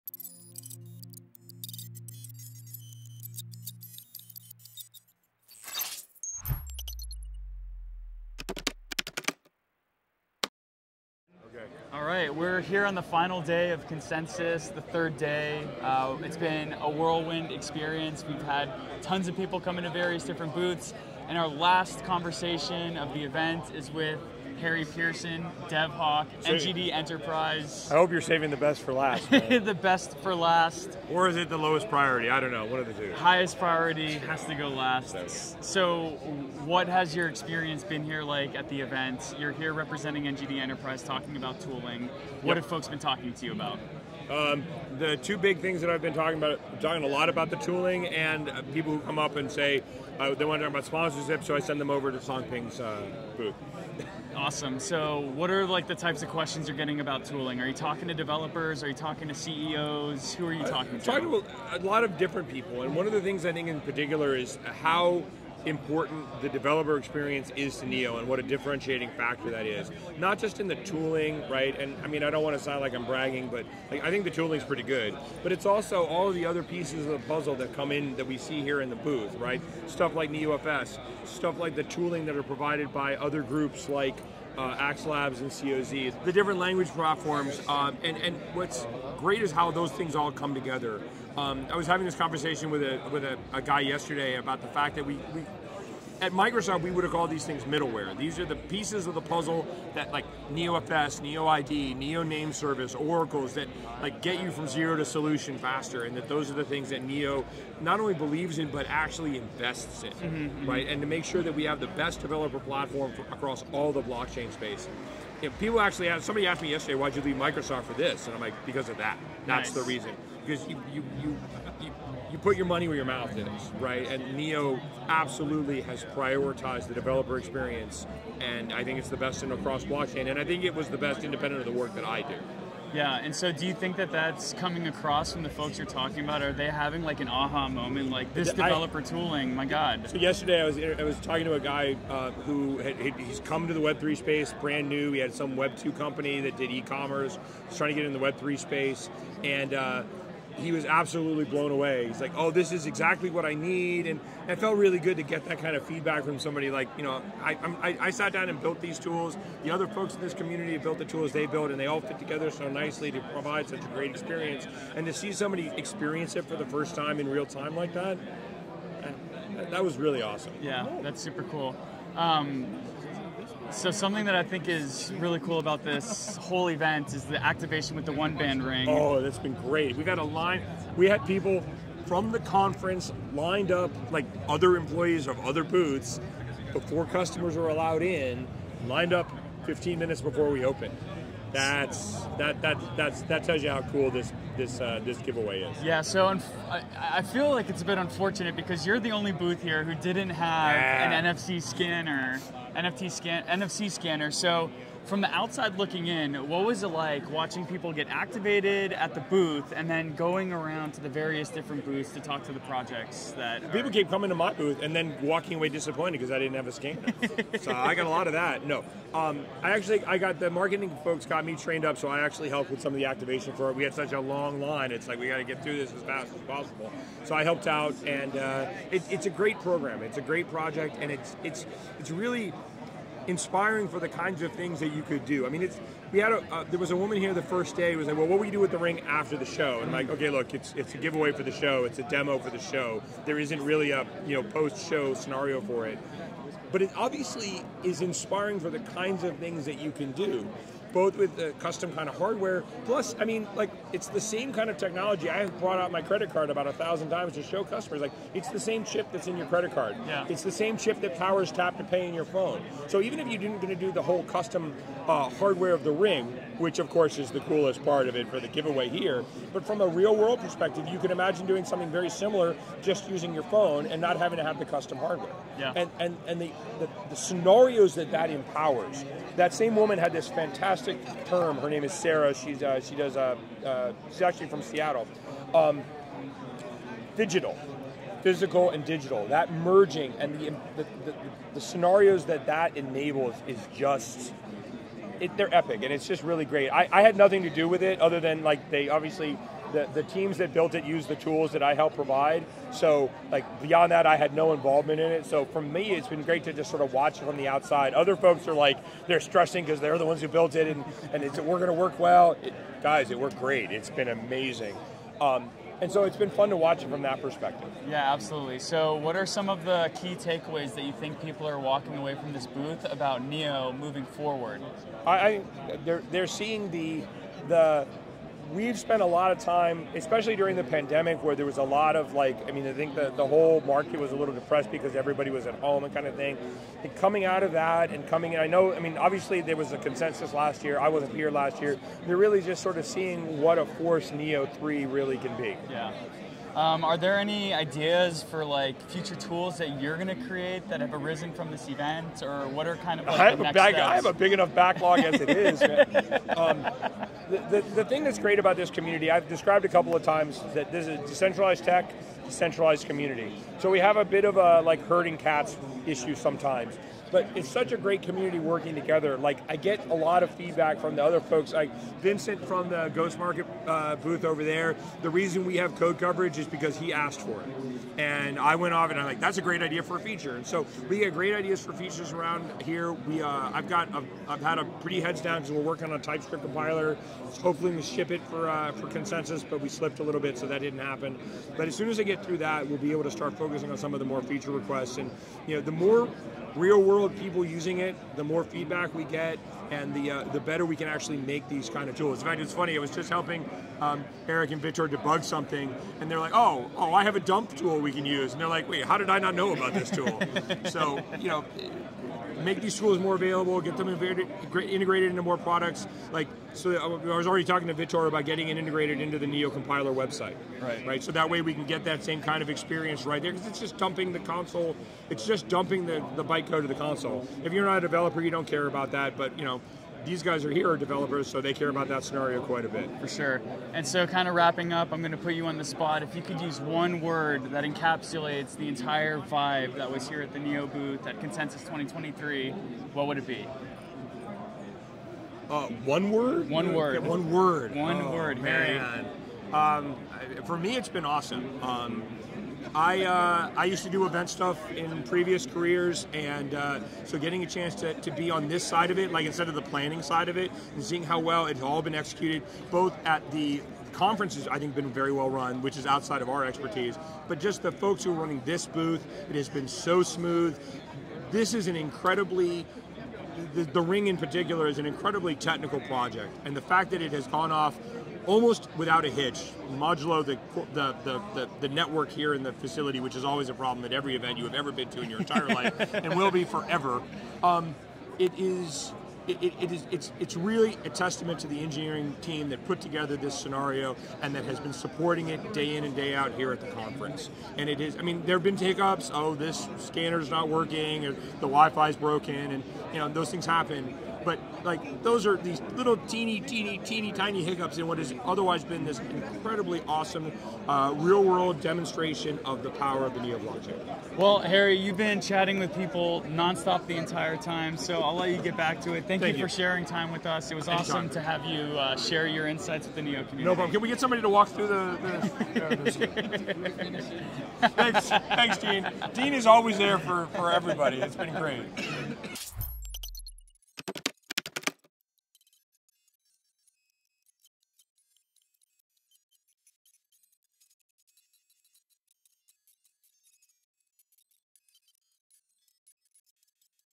Neo at Consensus 2023 | Interview with Harry Pierson, NGD Enterprise
Dylan Grabowski of NNT interviews NGD Enterprise chief architect Harry Pierson on developer tools, oneband onboarding, and Neo community collaboration.